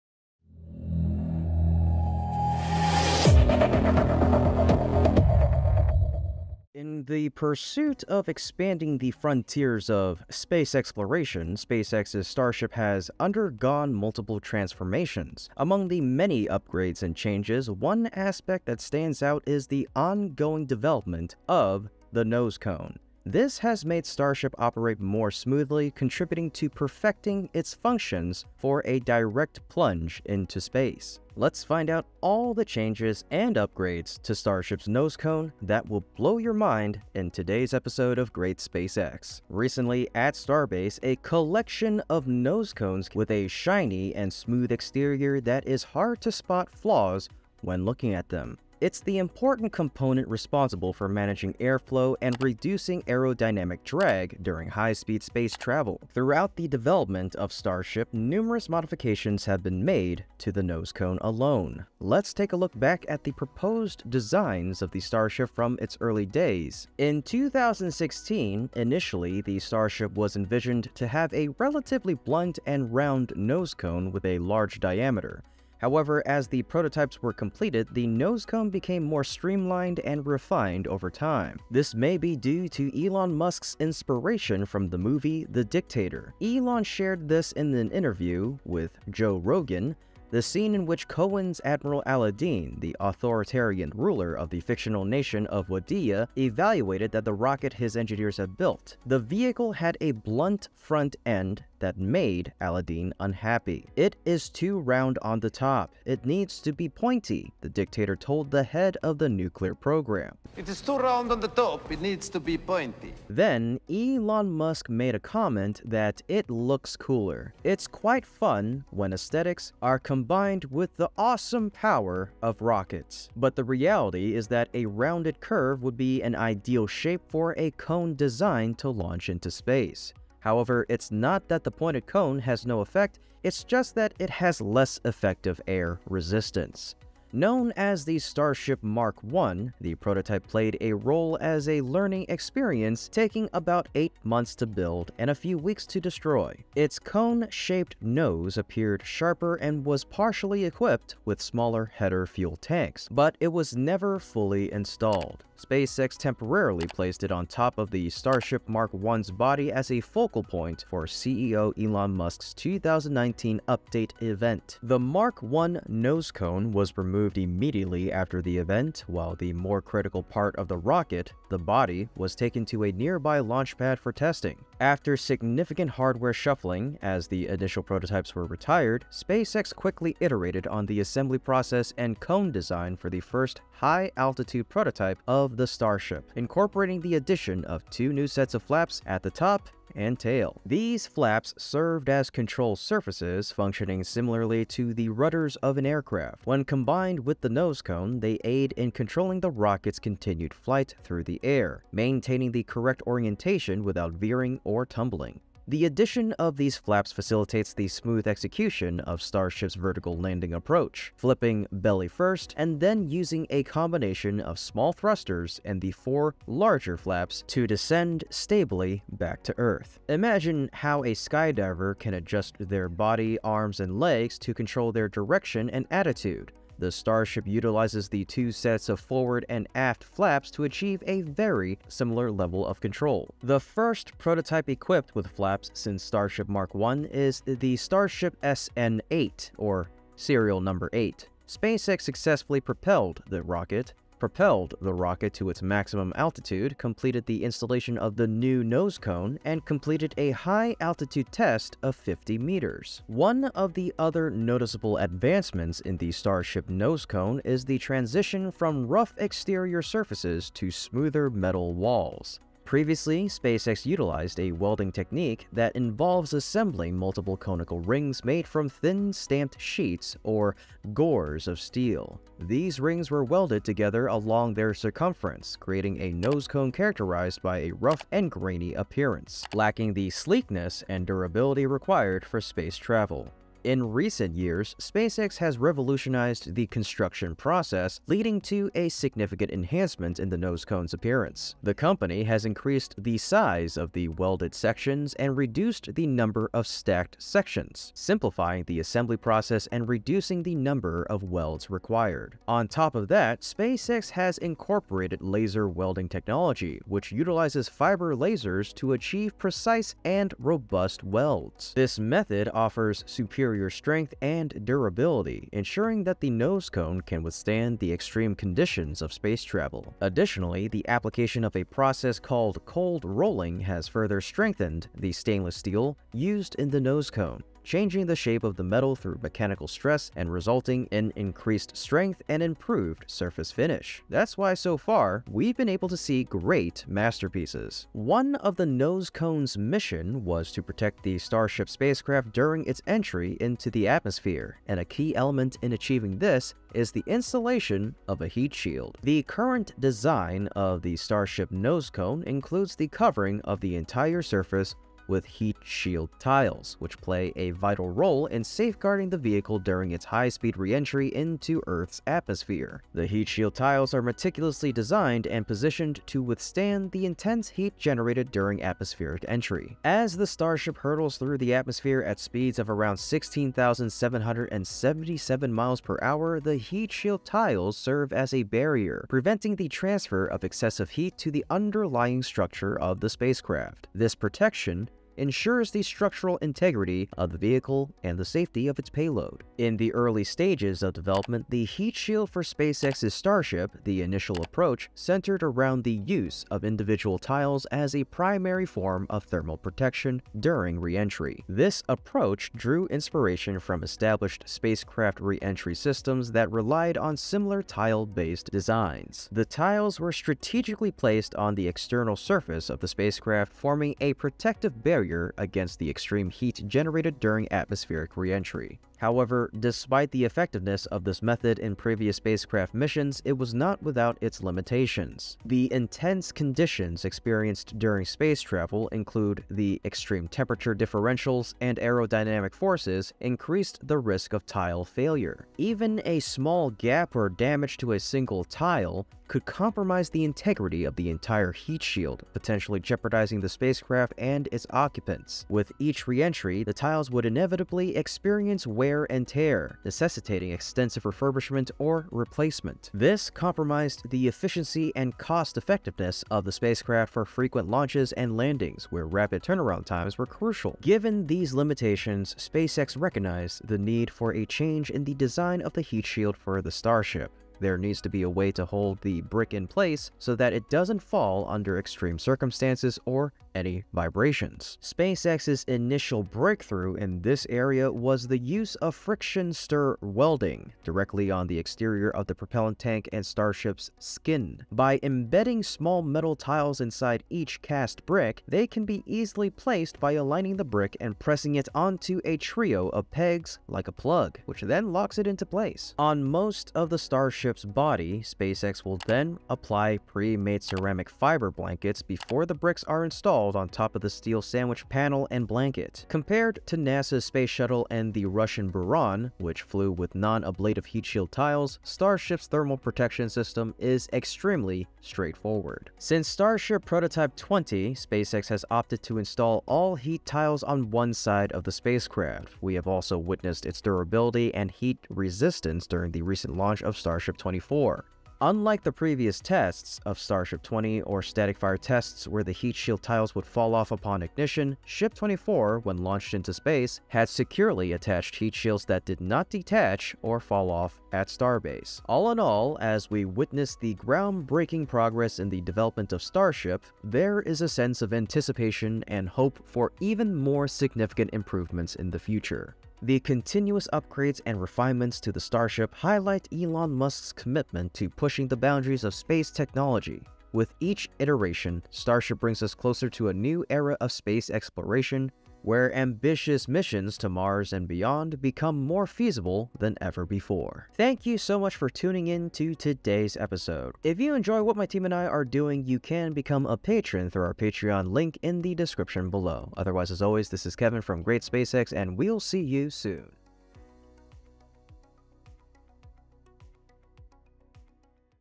Starship's Nosecone design change will blow your mind...
1) 🎵 ABOUT ALL TRACK IN MY VIDEO 🎵

The Changes in Starship's Nosecone will blow your mind...
In the pursuit of expanding the frontiers of space exploration, SpaceX's Starship has undergone multiple transformations. Among the many upgrades and changes, one aspect that stands out is the ongoing development of the nosecone. This has made Starship operate more smoothly, contributing to perfecting its functions for a direct plunge into space.
Let's find out all the changes and upgrades to Starship's nose cone, that will blow your mind!
Recently, at Starbase, a collection of nose cones can be seen in front of tent door 2, with a shiny and smooth exterior that is hard to spot flaws when looking at them. It is the important component responsible for managing airflow and reducing aerodynamic drag during high-speed space travel.
Throughout the development of Starship, numerous modifications have been made to the nose cone.
Let's take a look back at the proposed designs of the Starship from its early days ITS 2016. Initially, the Starship was envisioned to have a relatively blunt and round nose cone with a large diameter.
However, as the prototypes were completed, the nose cone became more streamlined and refined over time.